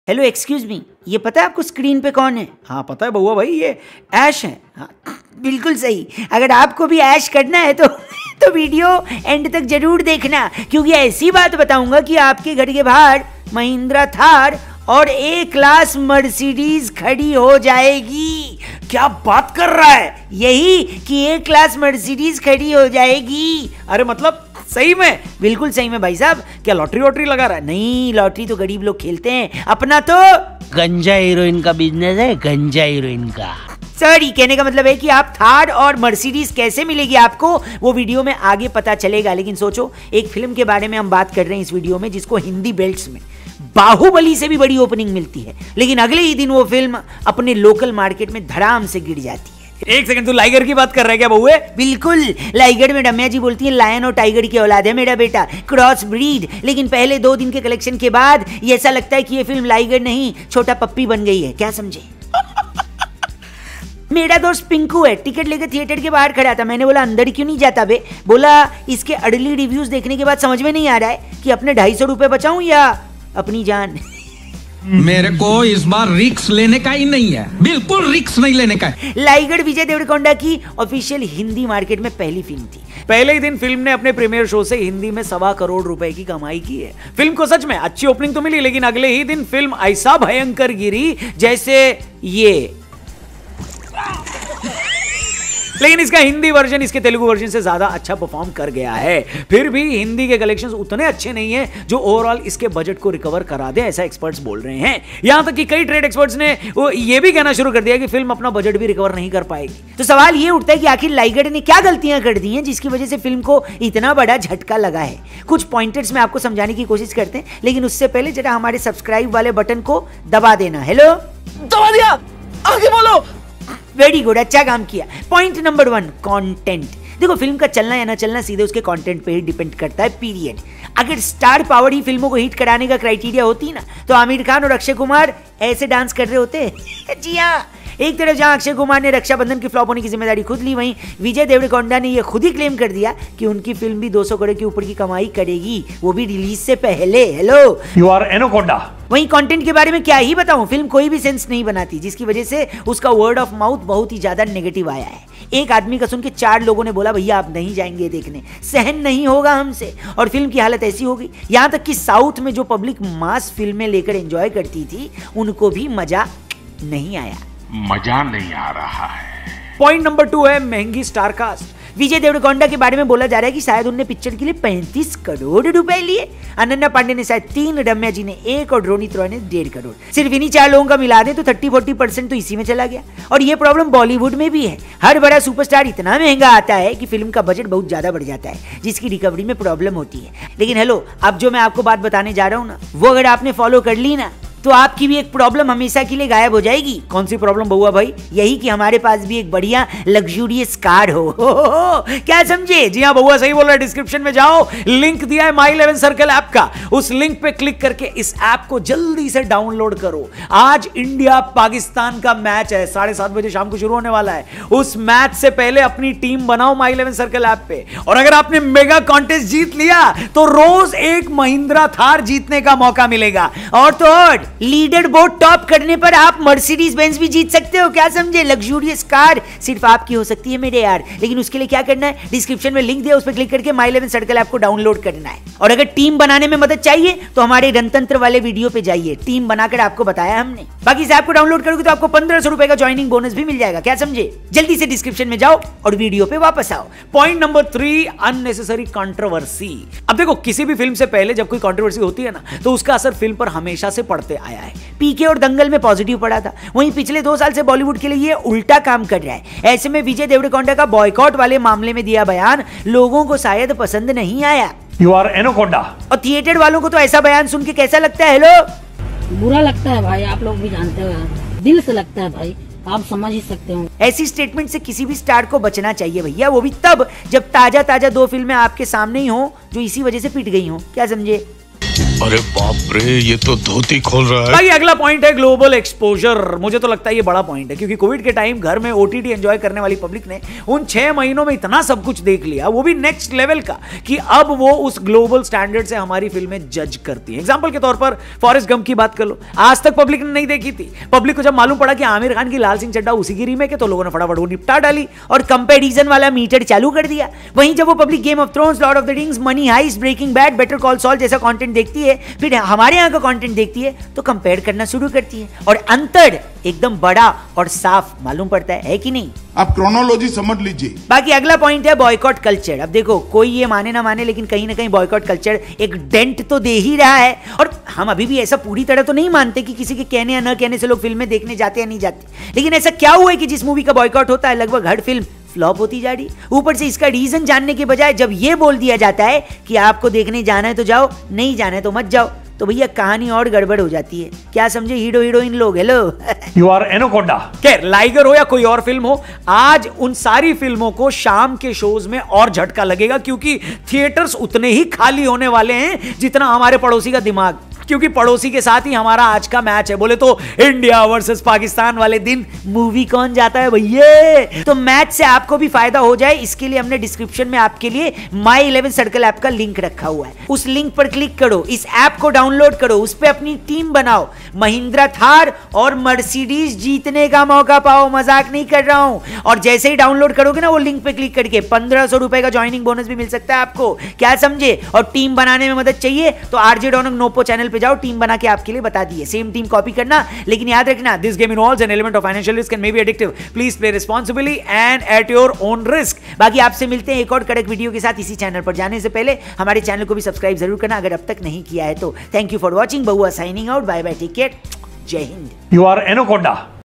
क्यों नहीं चली Liger? किसकी गलती? | Vijay Devarakonda | Ananya Pandey | RJ Raunak
My11Circle App Link :
Use my exclusive coupon: YT2022 to get additional 20% extra cash on first deposit